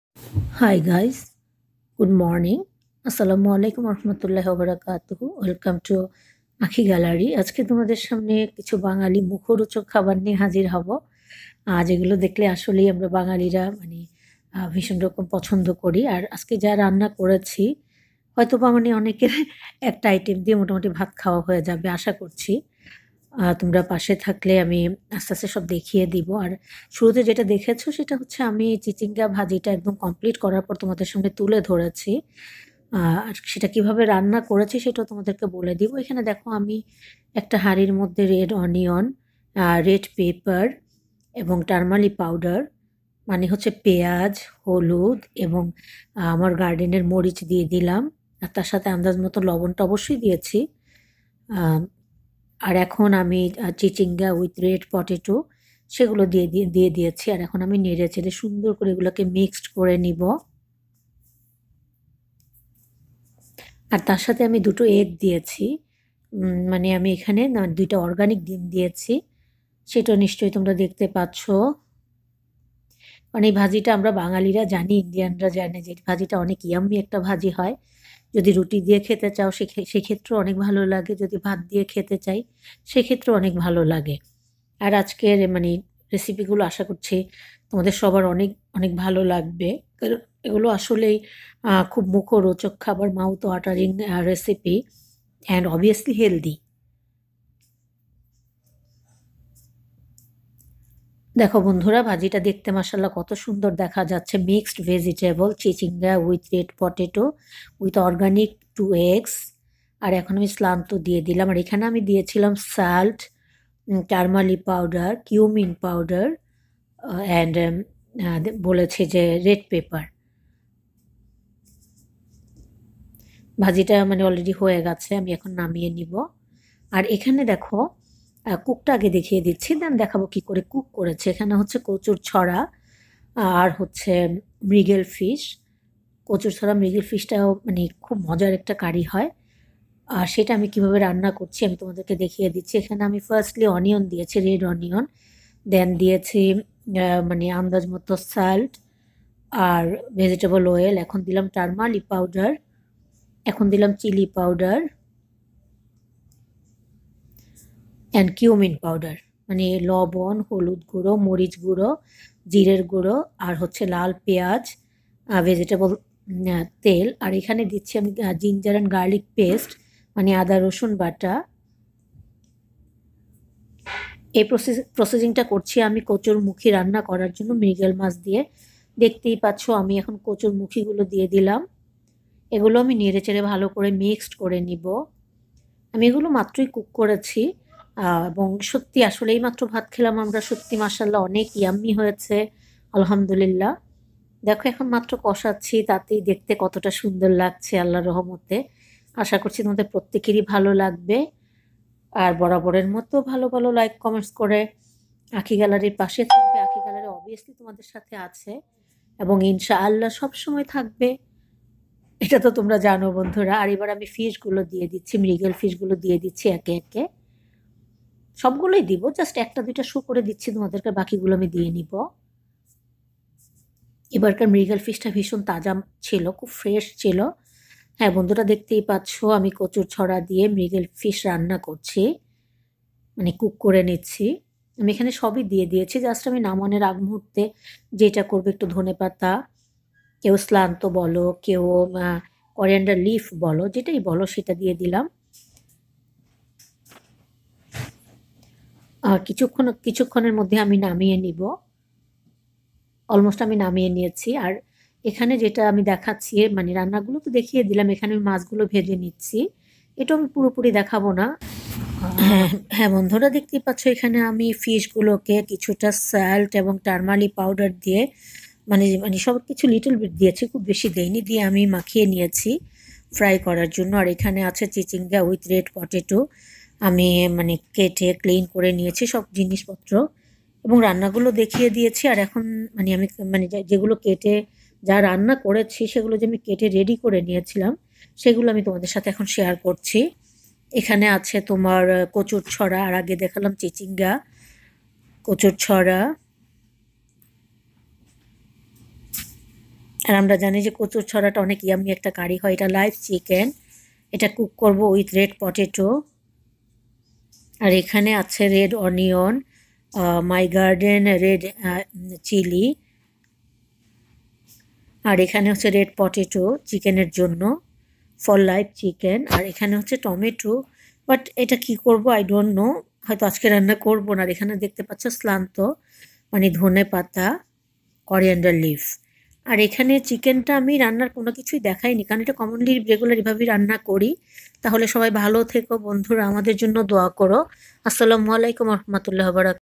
চিচিংগা আলু ও ডিমের মজার রেসেপি/ Chichinga, Potato and Organic egg Yeammy vegetables
Chichinga,  Potato and two egg one Yeammy and healthy recipe.  2 ) Mrigel Fish with Aaram one delicious Curry  3) Live Chicken.  Everything is very tasty and healthy food Guys. 😋😋😋😋. Thanks all My You Tube friends.